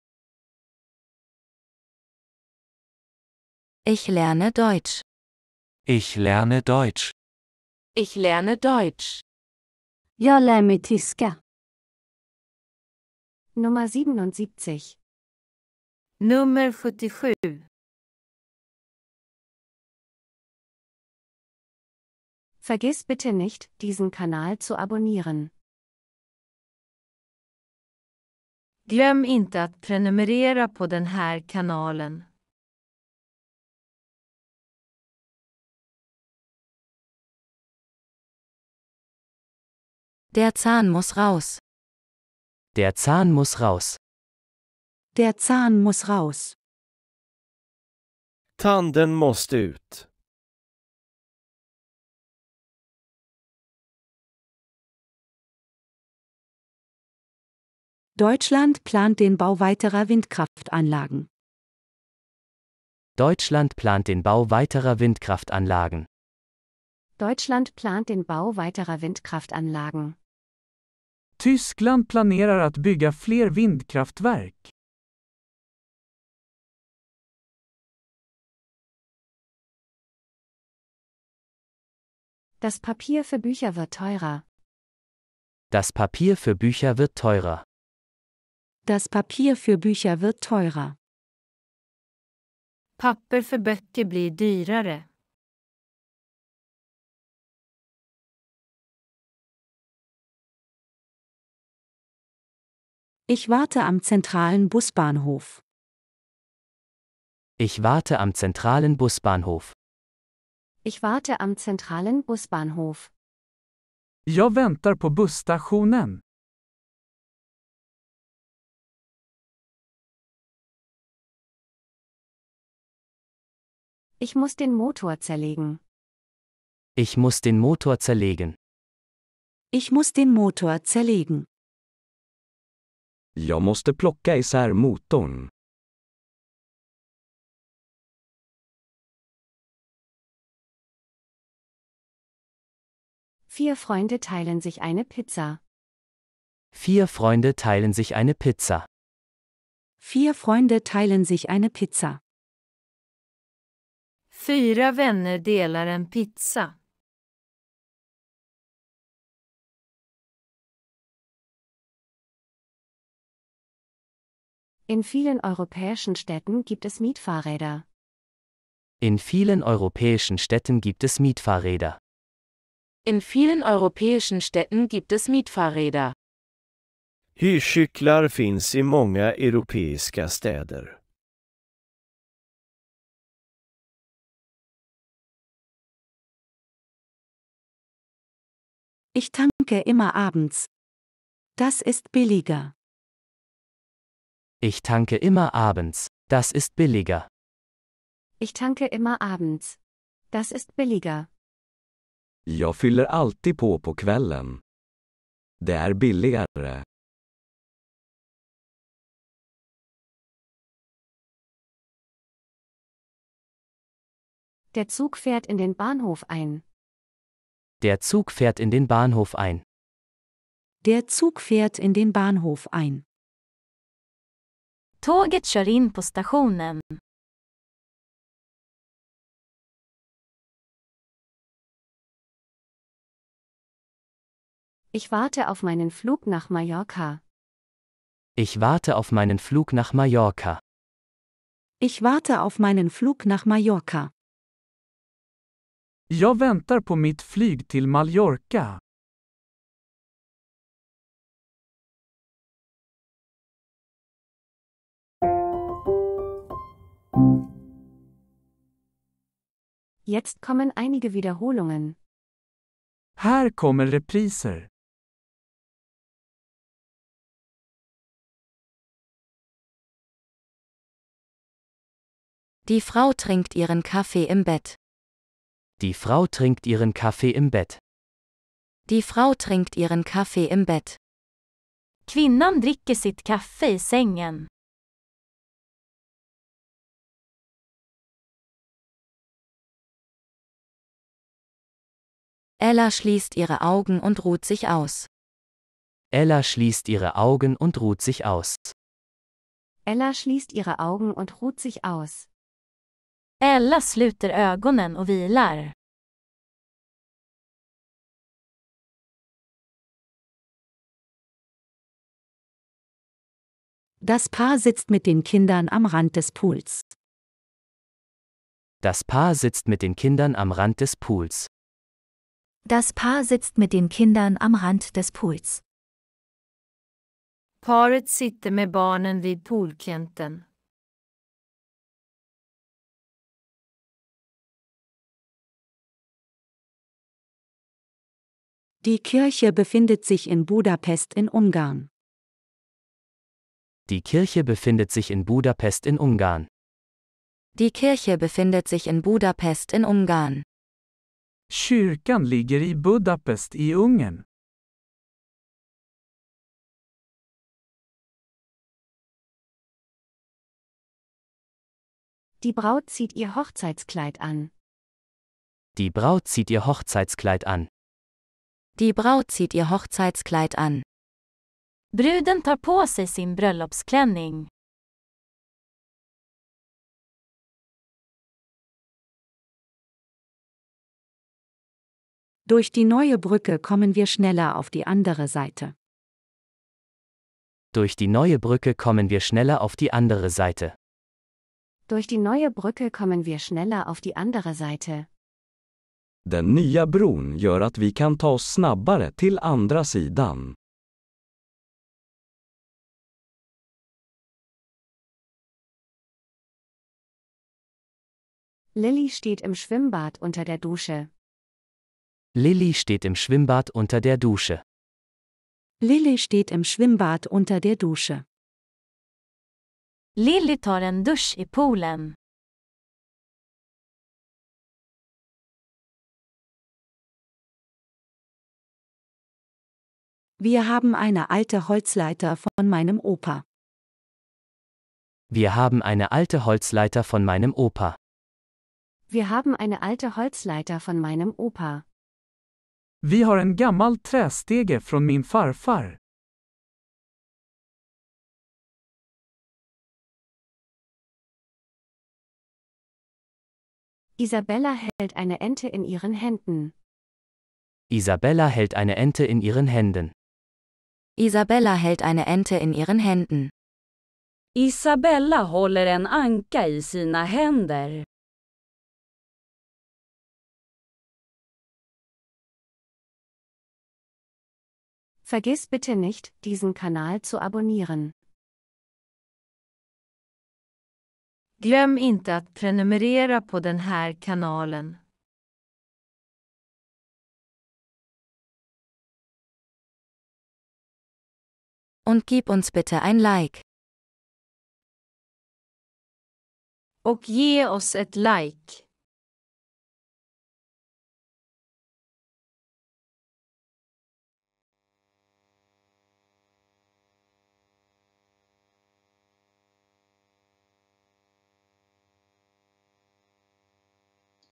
⭐77⭐Jag lär mig tyska med svensk översättning. Ich lerne deutsch mit schwedischer Übersetzung.
Det kommer en ny video varje dag klockan 14.00. Jeden Tag um 14 Uhr gibt es ein neues Video.
03 Der Zahn muss raus!
04 Deutschland plant den Bau weiterer Windkraftanlagen.
05 Das Papier für Bücher wird teurer.
06 Ich warte am zentralen Busbahnhof.
07 Ich muss den Motor zerlegen.
08 Vier Freunde teilen sich eine Pizza.
09 In vielen europäischen Städten gibt es Mietfahrräder.
10 Ich tanke immer abends. Das ist billiger.
11 Der Zug fährt in den Bahnhof ein.
12 Ich warte auf meinen Flug nach Mallorca.

⭐⭐⭐⭐⭐ 70+ VIDEOS
👀 Alle verfügbaren Videos mit schwedischer Übersetzung: